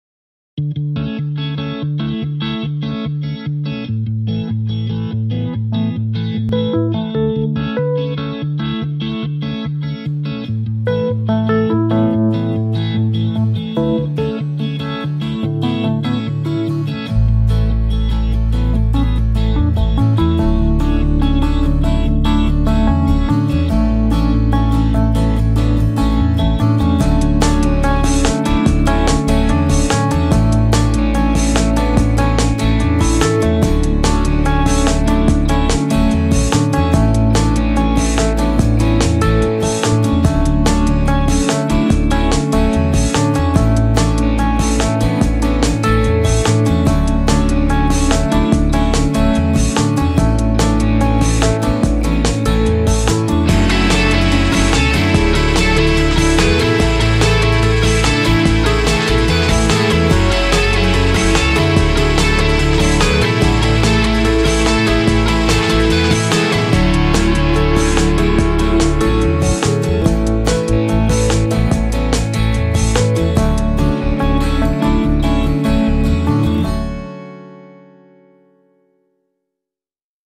Monitoimityökalu Ryobi RMT200-S 200W
Videolla esitellään Ryobin 200W monitoimityokalua.